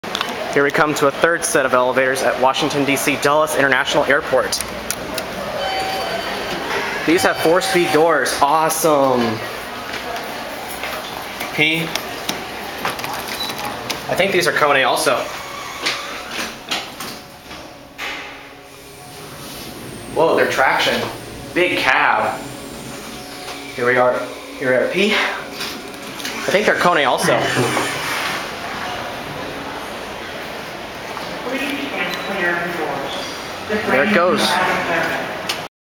2nd Kone Traction Elevators At Washington DC Dulles Airport
Riding the 3rd elevators at Washington DC Dulles Airport